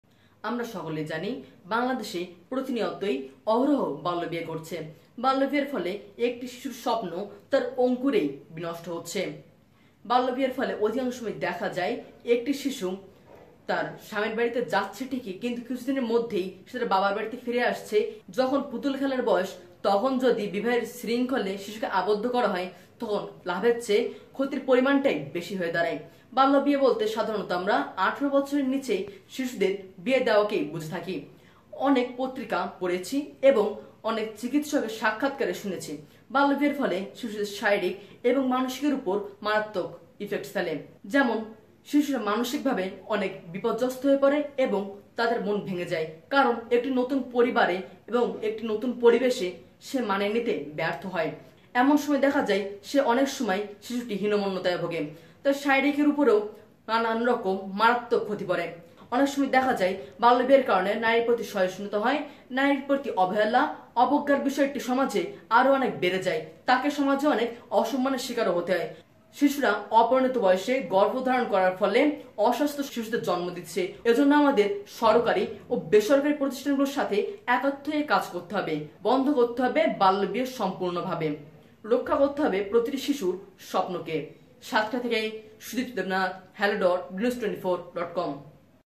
বাল্যবিয়ে: শিশুর স্বপ্নের পথে বাধা
বাল্যবিয়ের ফলে একটি শিশুর মানসিক ও শারীরিক স্বাস্থ্যে প্রভাব পড়ে। নষ্ট হয়ে যেতে পারে তার জীবন।

বাল্যবিয়ের ফলে একজন কিশোরীর মৃত্যুঝুঁকিও তৈরি হয়। মানসিকভাবে সে পুরোপুরি ভেঙে পড়তে পারে। কারণ একজন শিশুর পক্ষে একটা নতুন পরিবারে নিজেকে মানিয়ে নেওয়া বেশ কষ্টকর একটা কাজ। কিন্তু আমরা এগুলো বোঝার চেষ্টা করি না।